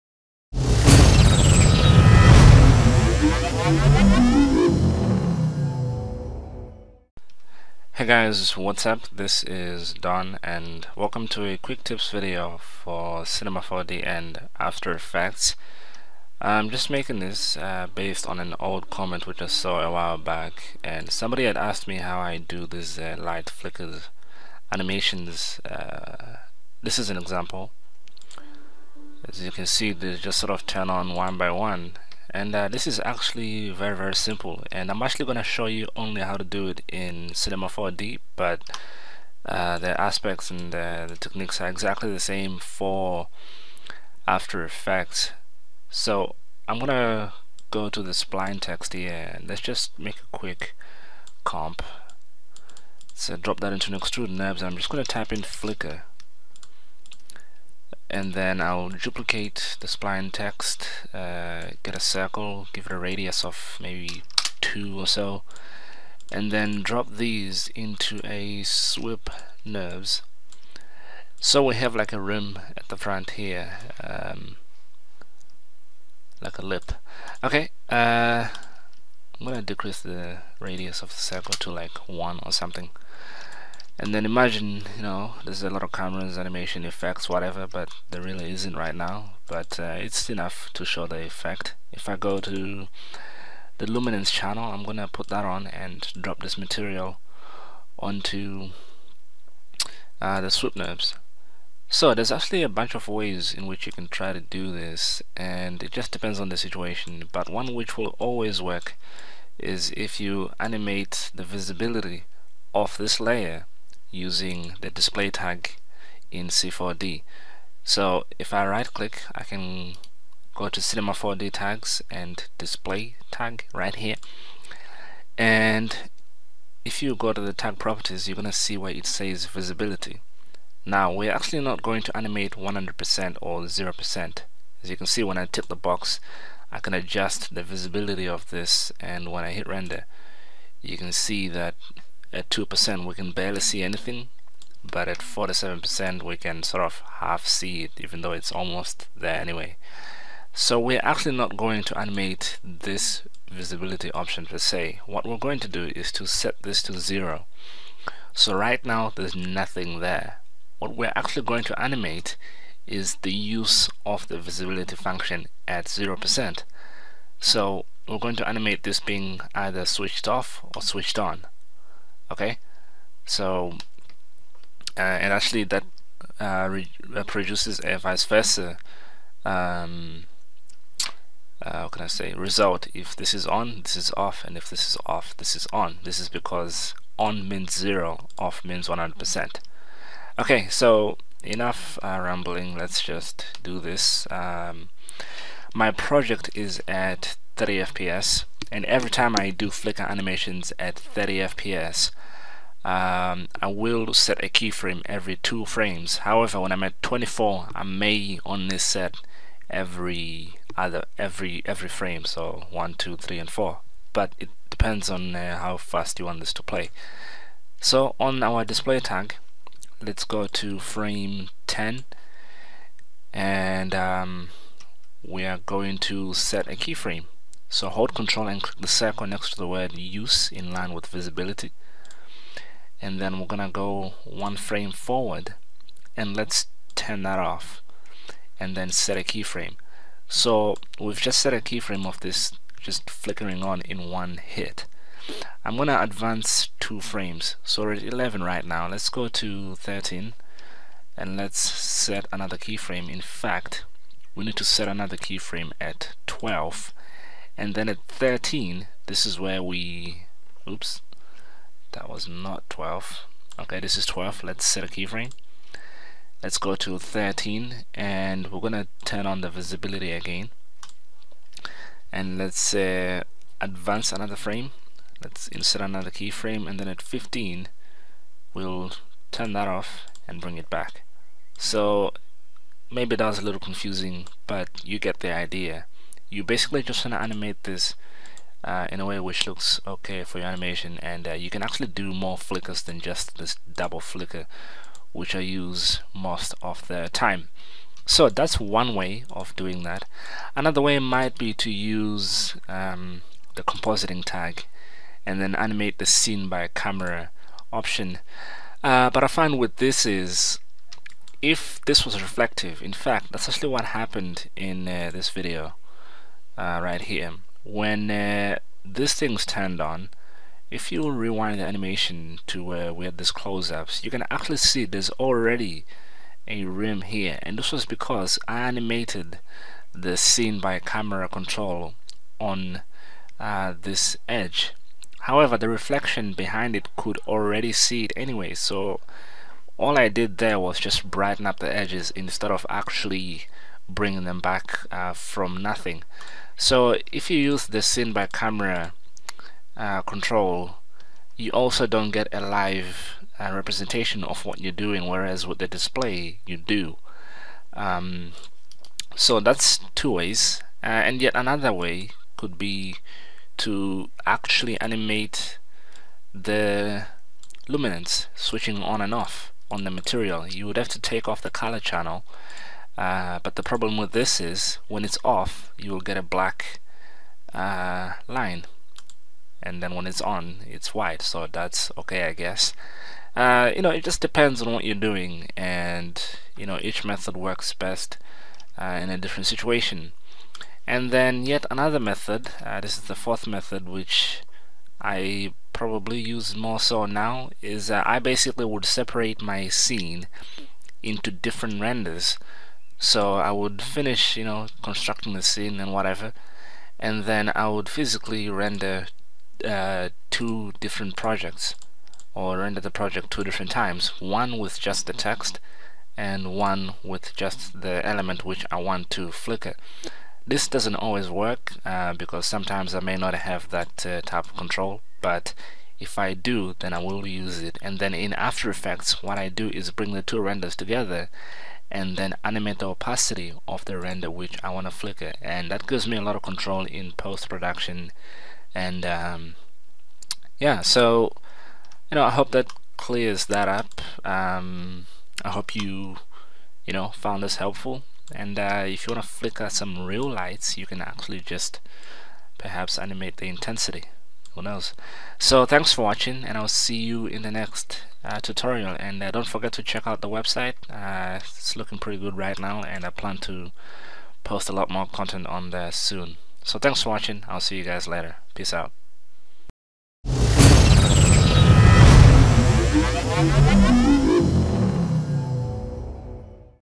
Cinema 4D: Light Flicker Effects
Learn how to create a flickering text effect using Cinema 4D. VideoFort Don shows you how to create flickering text by creating a text layer and dropping it into extrude NURBS. From there you duplicate the text, create a circle (approximate radius 2 cm), and drop both those into a sweep NURBS layer. Now you have a 3D text to add a flicker effect. From there you create a luminance layer and drop it on the sweep NURBS layer. To create the flicker effect you will turn on and off the sweep NURBS layer's visibility by setting keyframes in the timeline.